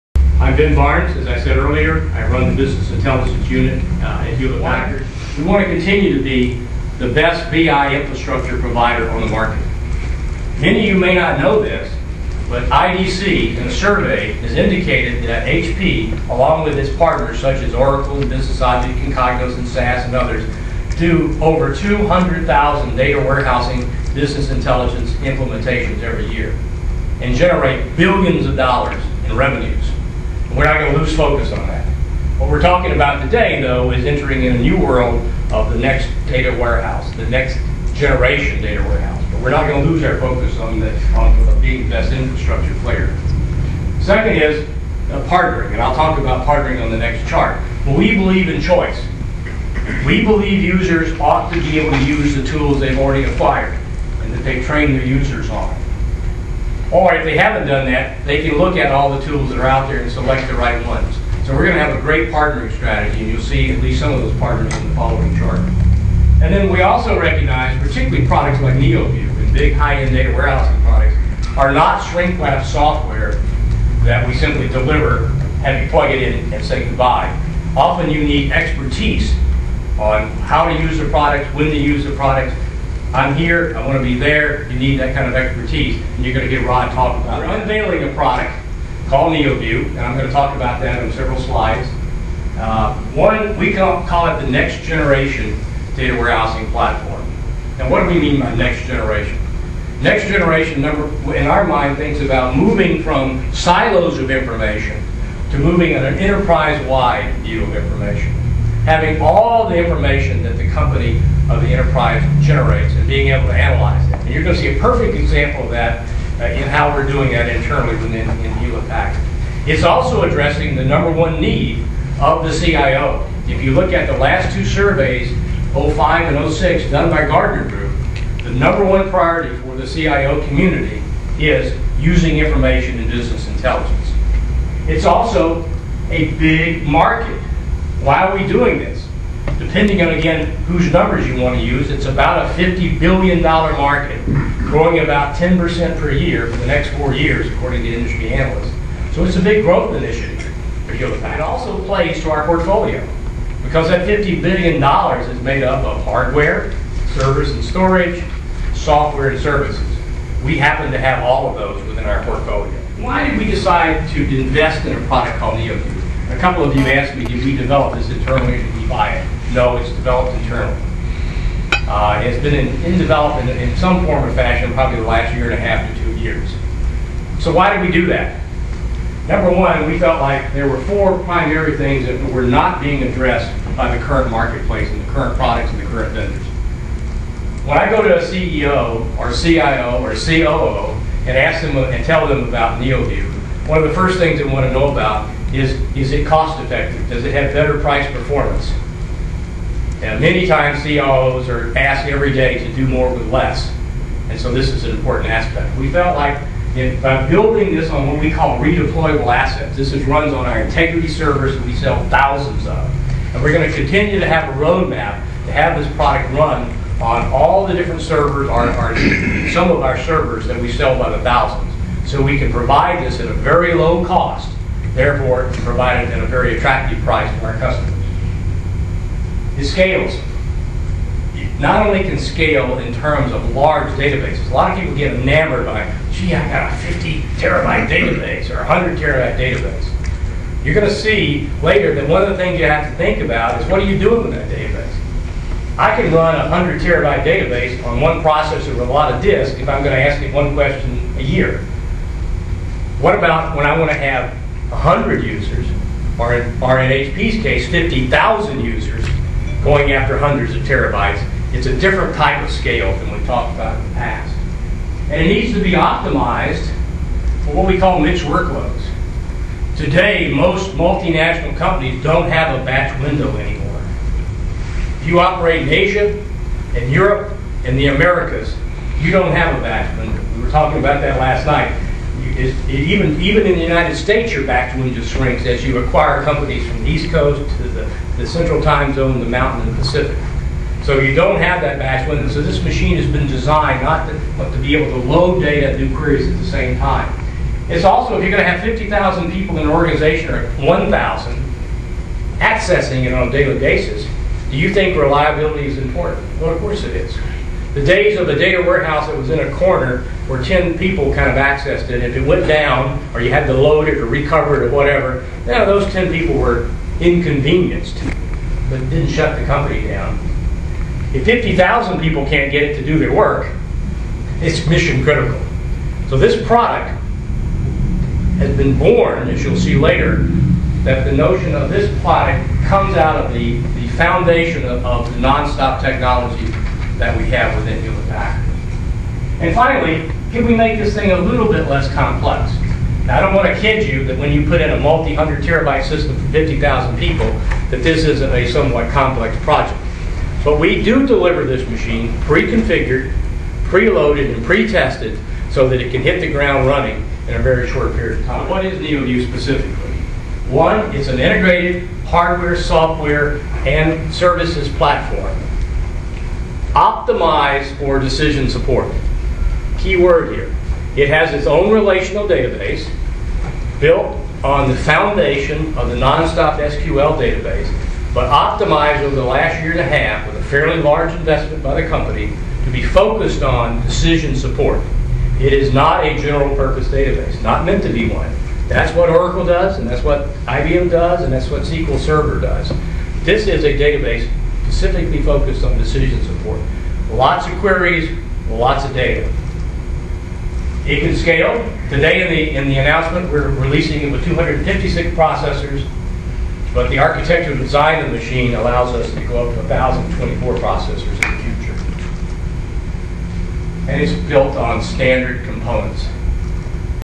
HP does 200,000 business intelligence implementations per ye
We just downloaded a short video of Ben Barnes, HP's VP & GM of Business Intelligence. Here are some of the highlights of his presentation:

HP is big in business intelligence:
We want to continue to be the best BI infrastructure provider [...] HP, along with his partners such as Oracle, Business Objects, Cognos, SAS and others, does 200,000 data-warehousing/business intelligence implementations every year and generate billions of dollars in revenues [...] And It's a big market: about a $50 billion a year growing 10%/year for the next 4 years [...] it also plays to our portfolio, because that $50 billion is made of hardware, servers, storage, software and services. And we happen to have all of those within our portfolio.

What's Neoview?
Neoview is the next generation of data-warehousing meaning moving from silos of information to an enterprise wide view of the information. Having all the information of the company generates and being able to analyse it [...] Neoview is also addressing the #1 need of the CIO i.e. using information in business intelligence [...] It's been in in development for the last 1.5-2 years [...] It's focused/optimised for decision support and not meant to be a general purpose database [...] Lots of queries, lots of data

Why did HP decide to develop Neoview internally?
Because there were 4 primary things that were not being addressed by the current marketplace, products and vendors...

- it's cost effective because built on a redeploy-able assets (i.e. Integrity servers that we sell by the thousands and can provide at a very low cost)

- scalable and optimised for mix workloads (no more batch windows): load data and do queries at the same time

- reliable: the days of the data-warehouse that was in a corner where 10 people kind of access it and if it went down or you have to load it or recover or whatever. Well those 10 people were inconvenienced but it did not shut the company down. But if 50,000 can't do any work, it's mission critical

- less complex: we do deliver this machine pre-configured, pre-loaded and pre-tested so it can hit the ground and running in a very short period of time.